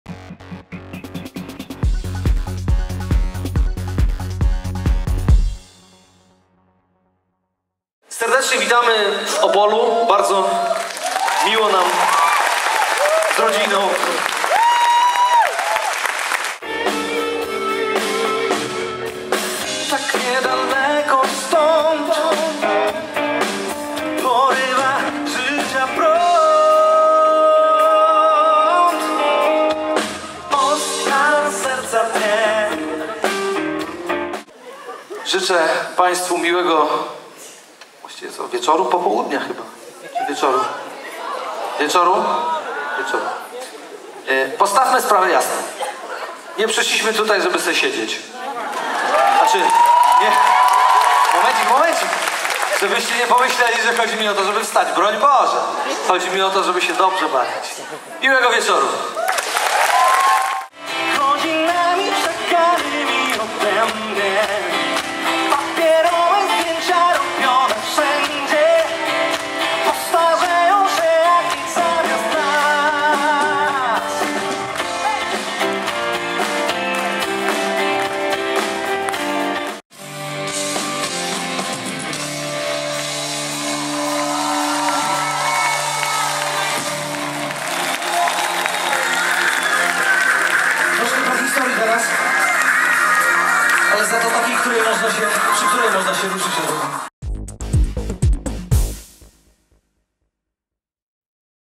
Andrzej Piaseczny w Opolu [nto.pl]
Andrzej Piaseczny w Opolu. Koncert w opolskim amfiteatrze.